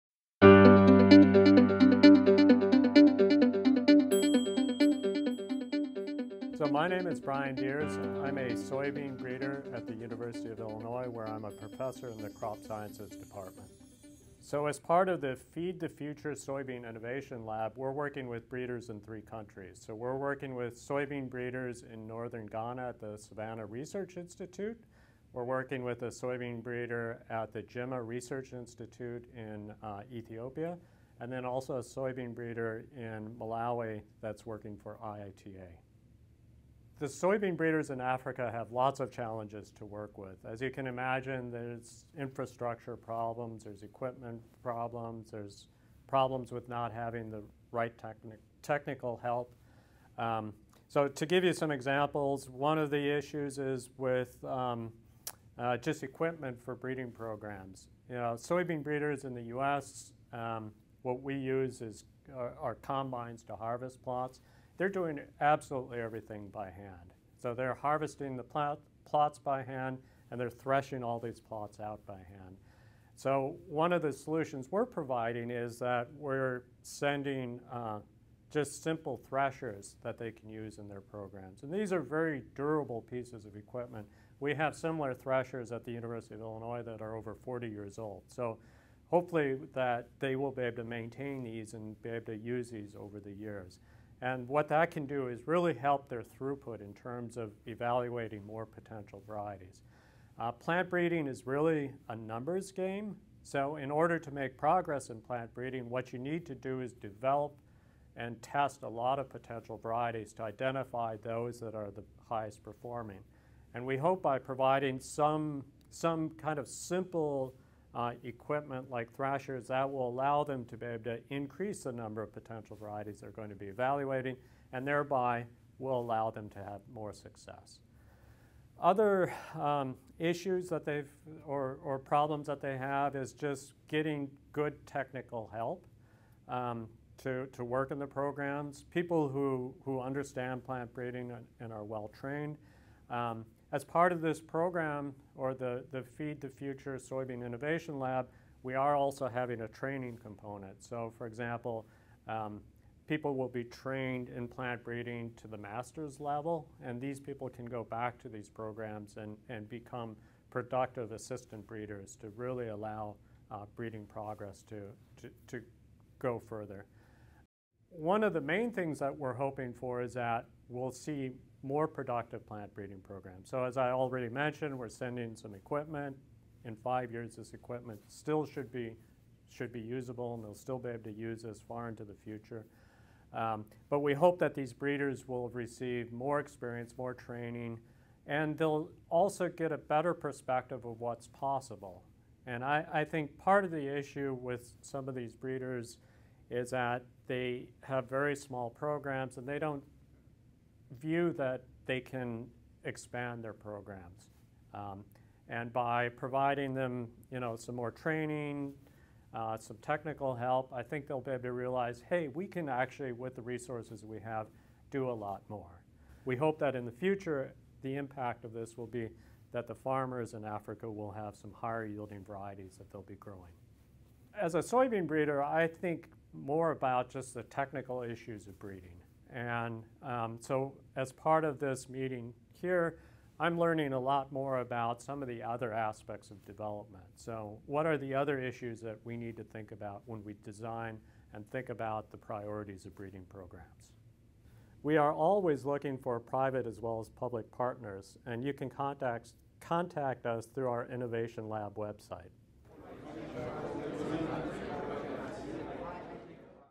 Workshop: Tropical Soybean for Development Workshop, Brian Diers
Dr. Brian Diers is a Professor of Crop Sciences at the University of Illinois. In this video, he describes some of the key challenges that plant breeders in Africa currently face. Dr. Diers conducts research on soybean germplasm and breeding for the Feed the Future Soybean Innovation Lab, and participated in the Tropical Soybean for Development Workshop on October 28.